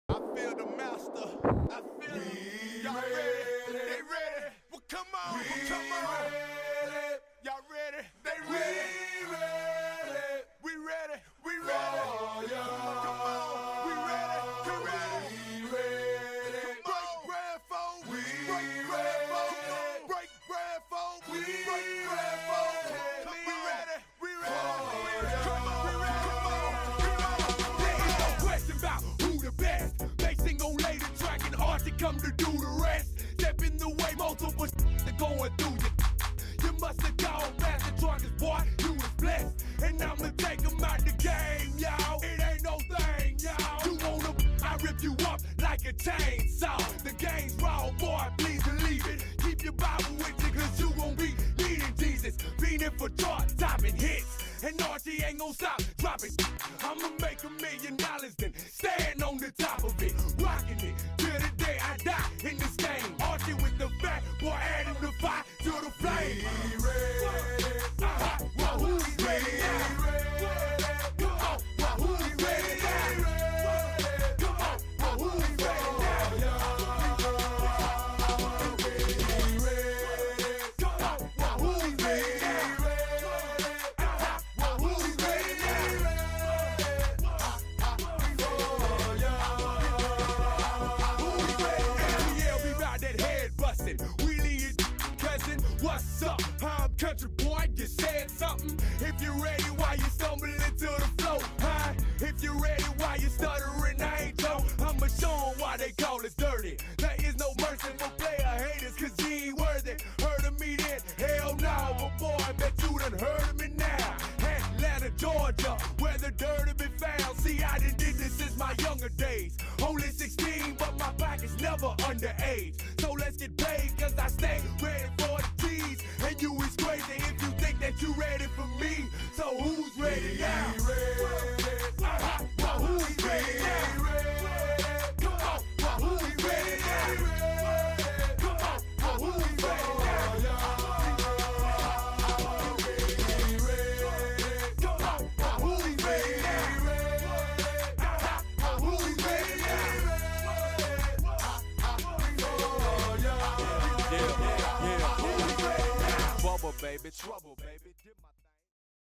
Ashton Ford, Kingsway Youth Football Highlights
Highlight film created by video pulled from game film found on Hudl.com.